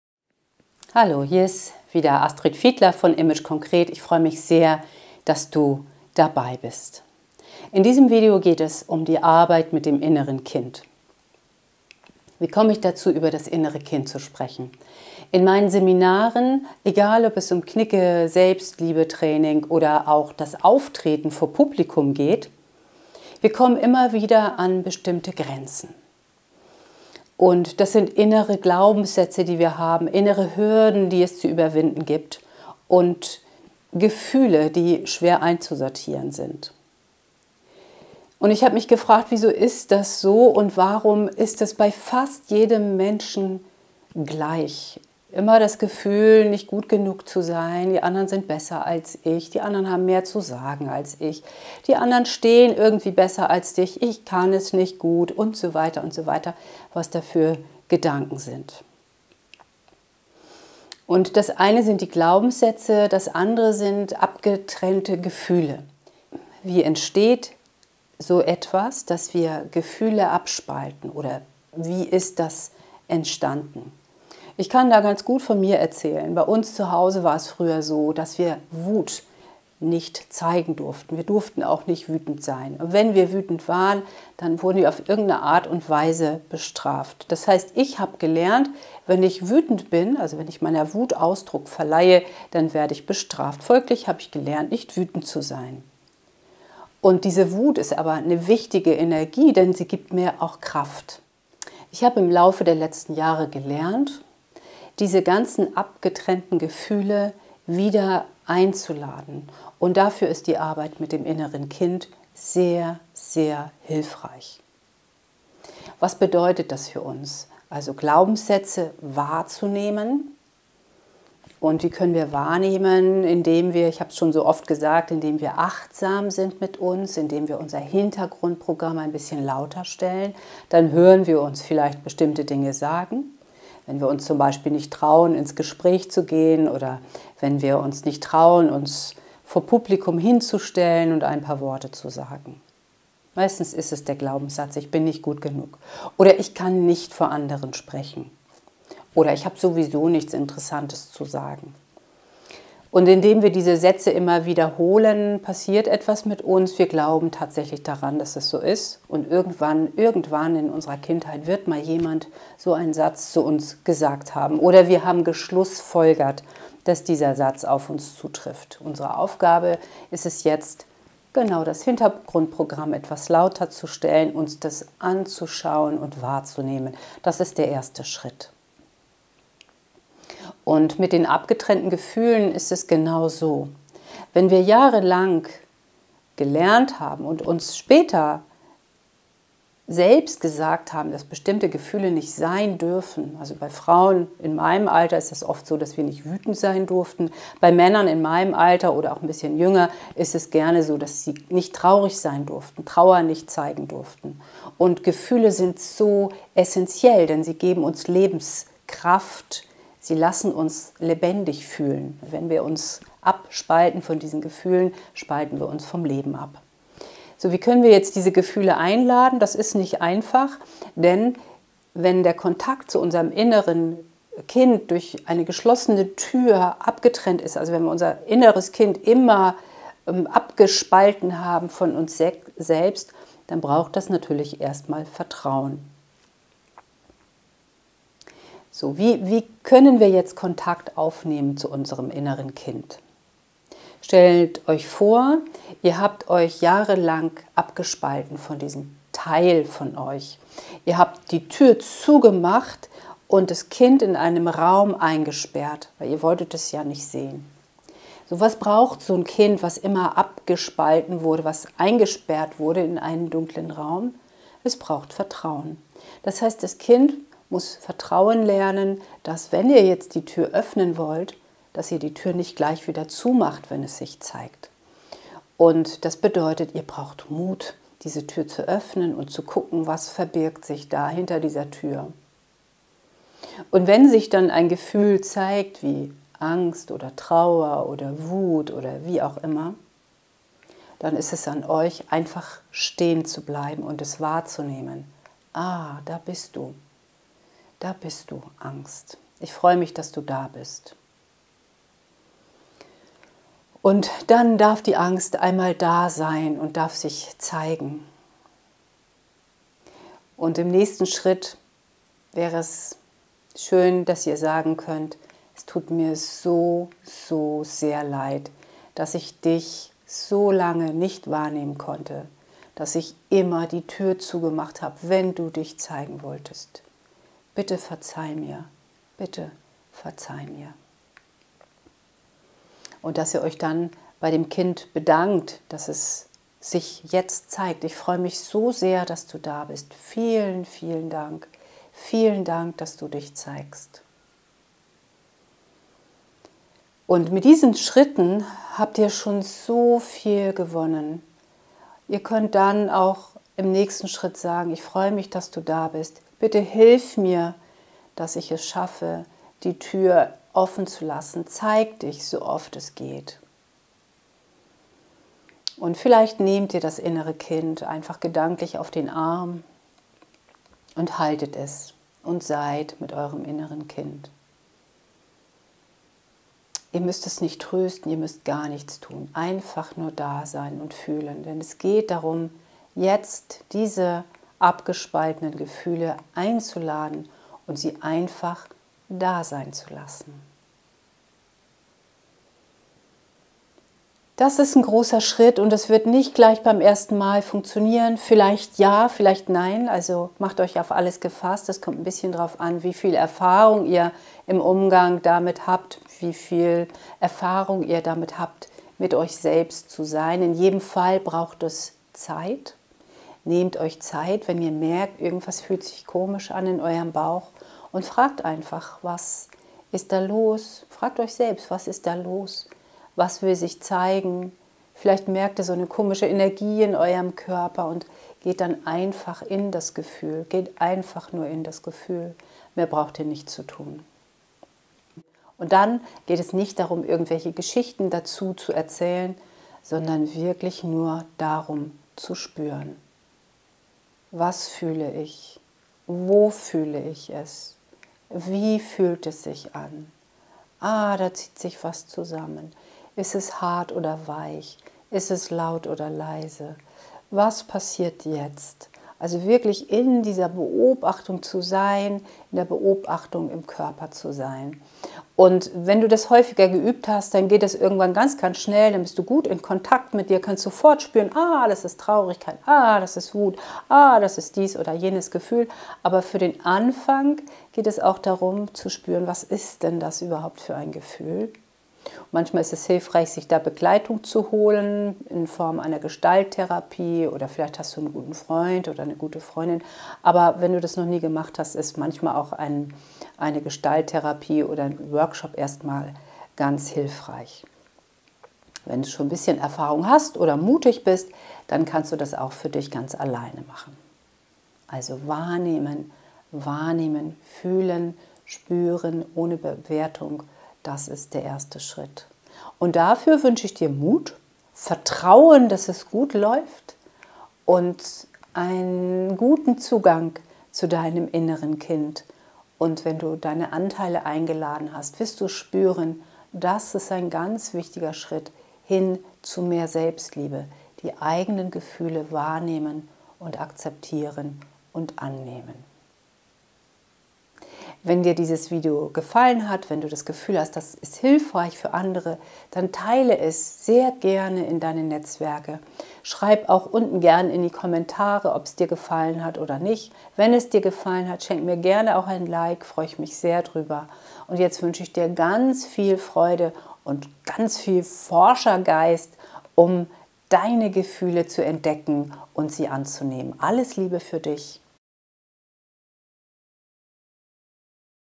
So heilst du dein inneres Kind und empfindest mehr Selbstbewusstsein und Selbstliebe
In diesem Video findest du Impulse dafür, wie du dein inneres Kind heilen und zu mehr Selbstbewusstsein und Selbstliebe finden kannst.

Schreibe gerne in die Kommentare, welche Erfahrungen du mit der Übung gemacht hast.

Ich wünsche dir von Herzen alles Gute.
Astrid

Alle Infos zu meinen Seminaren und zu Vorträgen findest du